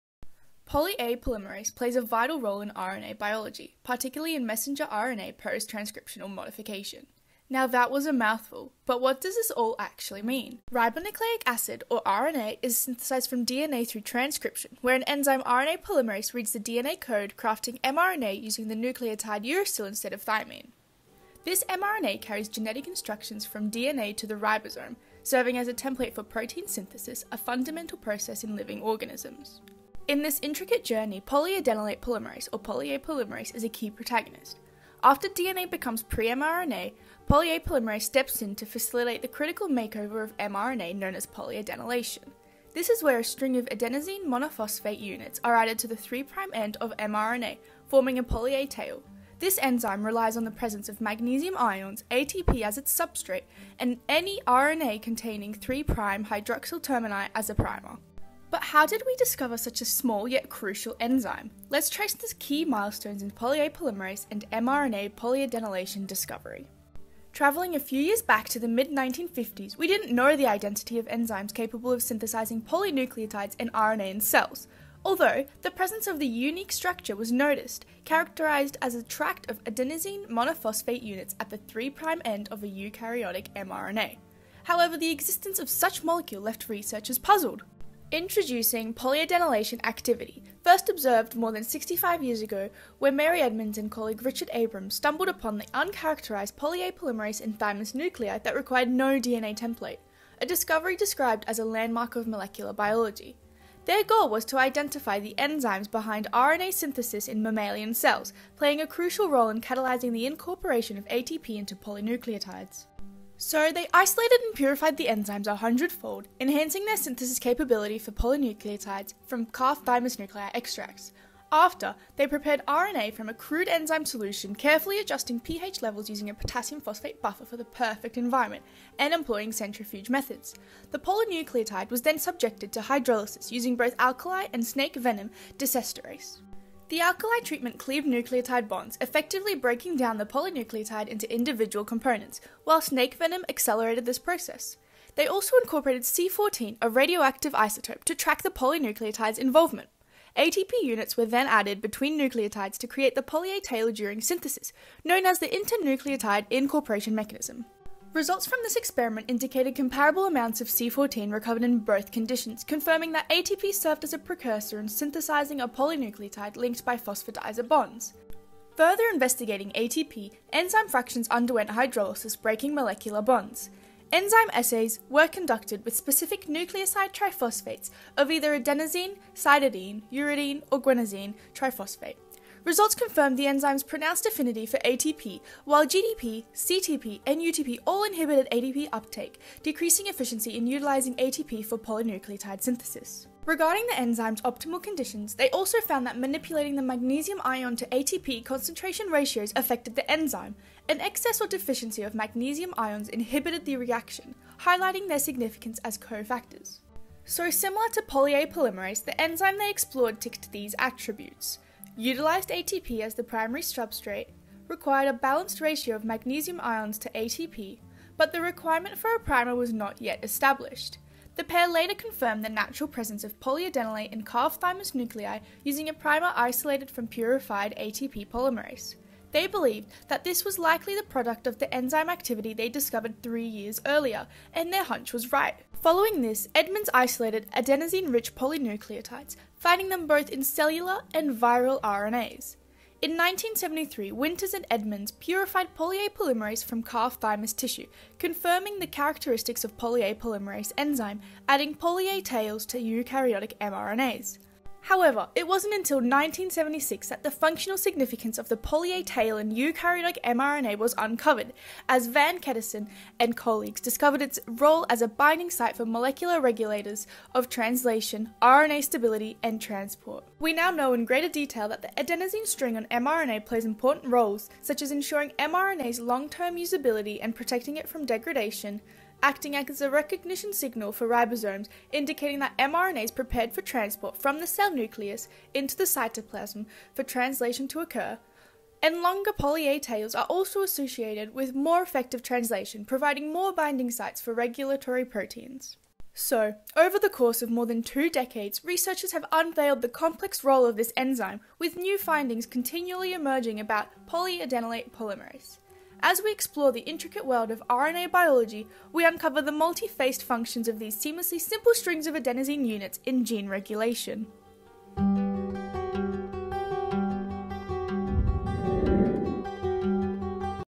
The discovery of polyadenylate polymerase: Unravelling the mysteries of molecular biology
An animated yet educational video that discovers the fascinating world of polyadenylate polymerase and mRNA polyadenylation. The indispensable role of Poly(A) polymerase, a pivotal enzyme crucial for messenger RNA post-transcriptional modification, is explored. This video dives into the intricate process of polyadenylation, giving rise to a poly(A) tail. Historical milestones of poly(A) polymerase and mRNA polyadenylation discovery are traced, from its enigmatic beginnings in the mid-1950s to the significant breakthroughs made by scientists like Mary Edmonds and Richard Abrams.

The story marks its major beginning in 1960, with the research of Edmonds and Abrams. Together, the delved into the incorporation of adenosine triphosphate (ATP) into polynucleotides with an enzyme from calf thymus nuclei. This process, known as the internucleotide incorporation mechanism, laid the foundation for many more discoveries to come. This initial experiment gave rise to the details of enzyme purification and provided information regarding some of the enzyme properties.

The video explores over two decades of pioneering experiments that isolated and purified poly(A) polymerase, while shedding light on its role in synthesising polynucleotides. Their results allowed us to gain insight on the enzyme’s preferences for ATP, magnesium ions, and primers in the synthesis process. This video culminates with a discussion of the protective, recognition, and translational roles of these seemingly simple strings of adenosine units in mRNA. Watch this video now to dive into an educational adventure into the world of molecular biology.

Creator: Natalie Katafiasz

References:
Mechanism of poly(A) polymerase: Structure of the enzyme-MgATP-RNA ternary complex and kinetic analysis. Balbo PB, Bohm A. Structure. 2007 Oct 17;15(9):1117-31. doi: 10.1016/j.str.2007.07.010.
Enzymatic synthesis of nucleic acid-like polynucleotides. Grunberg-Manago M, Ortiz PJ, Ochoa S. Science. 1955 Nov 11; 122(3176):907-10. doi: 10.1126/science.122.3176.907.
Polynucleotide biosynthesis: Formation of a sequence of adenylate units from adenosine triphosphate by an enzyme from thymus nuclei. Edmonds M, Abrams R. The Journal of Biological Chemistry. 1960 April 1; 235(4):1142-49. doi: 10.1016/S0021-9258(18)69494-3.
Isolation of a naturally occurring poly-adenylate from calf thymus nuclei. Edmonds M, Abrams R. The Journal of Biological Chemistry. 1963 Mar 1; 238(3):1186-87. doi: 10.1016/S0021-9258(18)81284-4.
The isolation and characterization of adenosine monophosphate-rich polynucleotides synthesised by Ehrlich ascites cells. Edmonds M, Caramela MG. The Journal of Biological Chemistry. 1969 Mar 10; 244(5):1314-24. doi: 10.1016/S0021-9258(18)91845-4.
Polyadenylic acid sequences in the virion RNA of poliovirus and Eastern Equine Encephalitis virus. Armstrong JA, Edmonds M, Nakazato H, Phillips BA, Vaughn MH.  Science. 1972 May 5;176(4034):526-8. doi: 10.1126/science.176.4034.526.
A poly(A) polymerase from calf thymus: Purification and properties of the enzyme. Winters MA, Edmonds M. The Journal of Biological Chemistry, 1973 Jul 10; 248(13):4756-62. doi: 10.1016/S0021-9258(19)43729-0.
Preparation and properties of a poly A resin and its use in the isolation of naturally occurring poly U sequence. Venkatesan S, Nakazato H, Edmonds M. Nucleic Acids Research.1976 Aug; 3(8):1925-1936. doi: 10.1093/nar/3.8.1925
Belfort M. (2014). Mary P. Edmonds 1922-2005. Biographical Memoirs: National Academy of Sciences, pp 2-5.